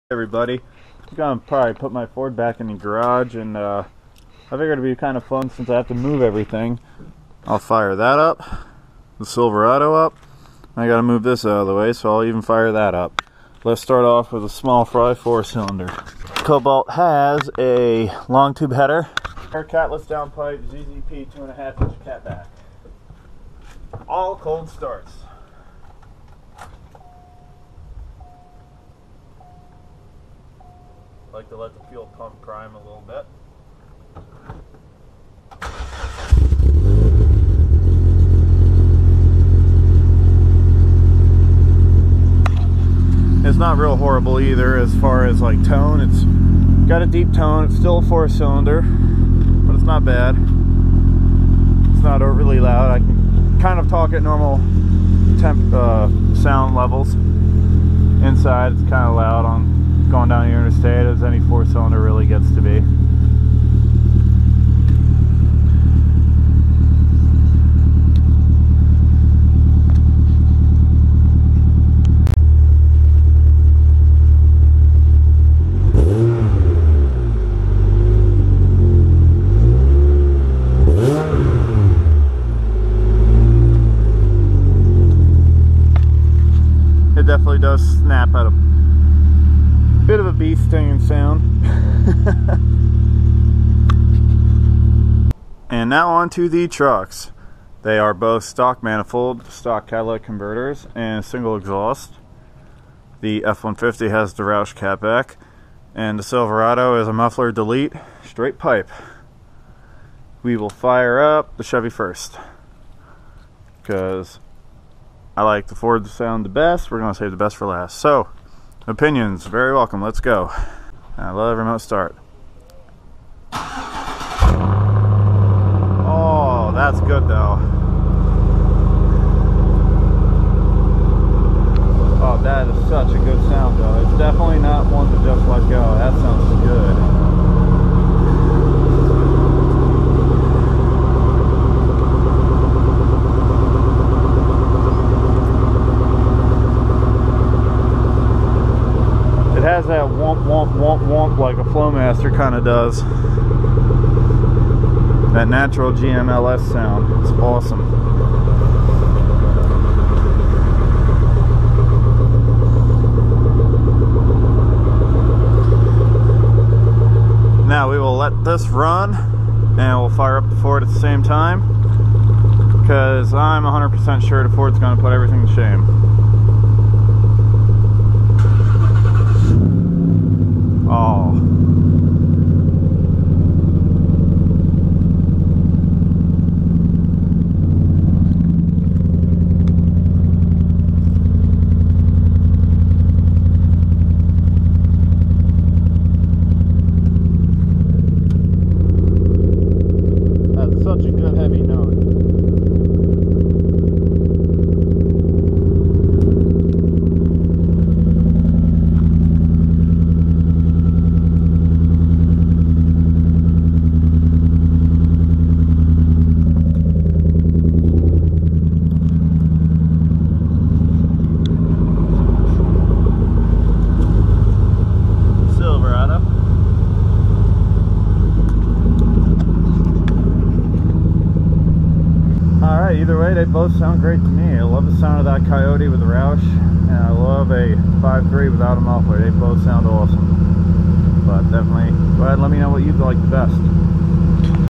Exhaust noise! F-150, Silverado, and even the Cobalt SS!
Lets here some noise!

2013 F150 5.0L: stock manifolds, stock cats, 3" Roush catback
2004 Silverado 5.3L: stock manifolds, stock cats, 3" single straight pipe
2005 Cobalt SS 2.0L: longtube header, catless downpipe, 2.5" ZZP exhaust.